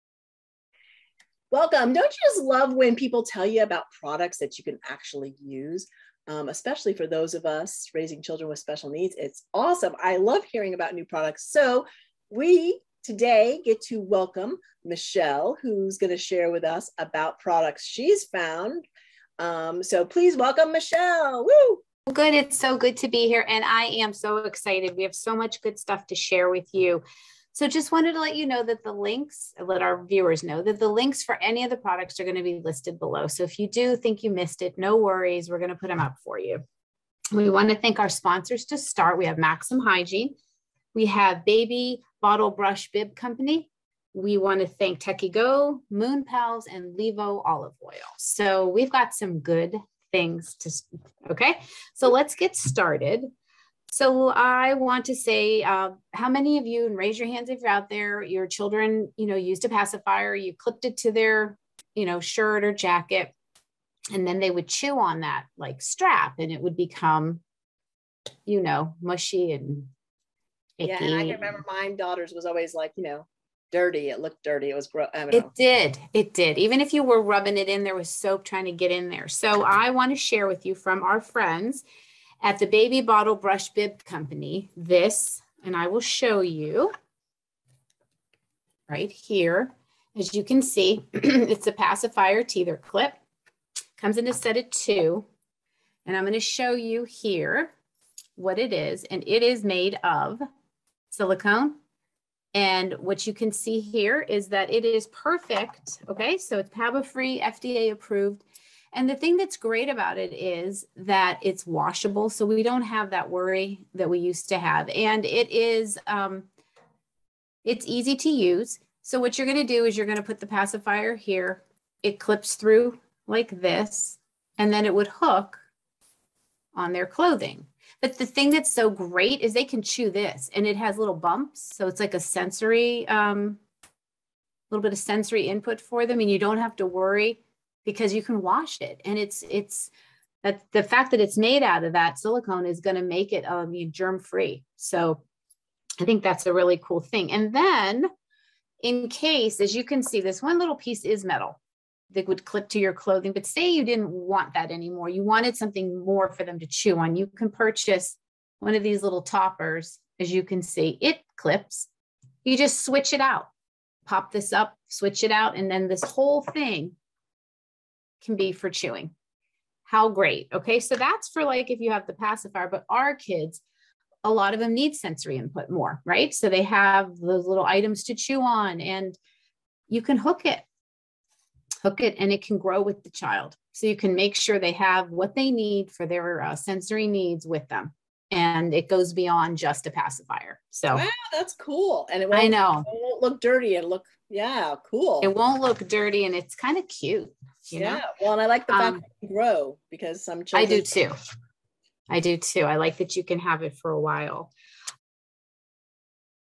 Parenting Special Needs Magazine and Our Product
Our pacifier Teether clips can be used as a sensory item for babies/children who have sensory needs